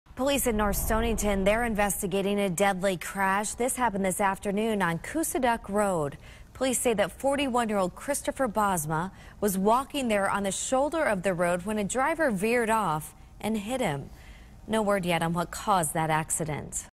Police identity person fatally hit by vehicle in N. Stonington
A pedestrian has passed away after being hit by a vehicle on Tuesday afternoon.